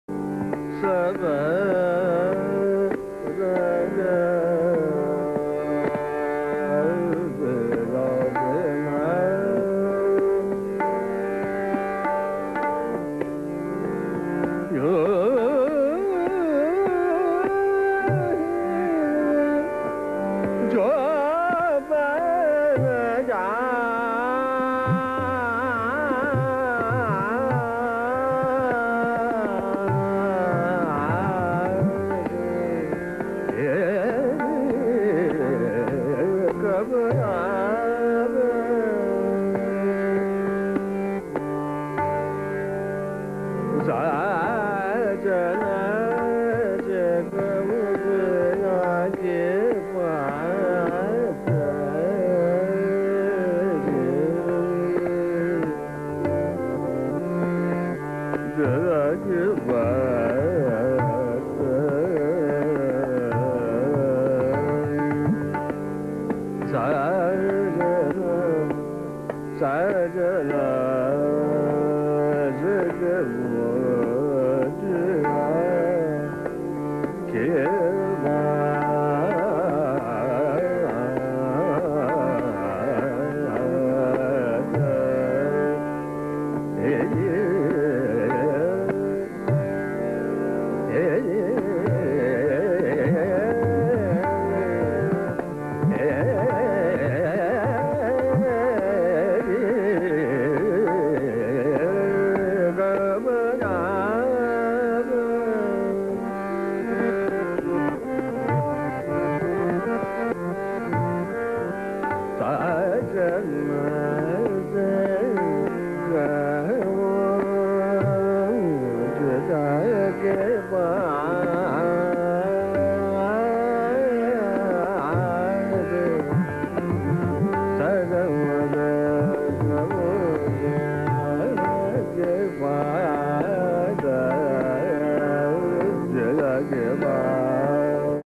Shantanu Bhattacharyya MalkaunsLive3rdPart
A masterly treatment of the majestic Raga Malkauns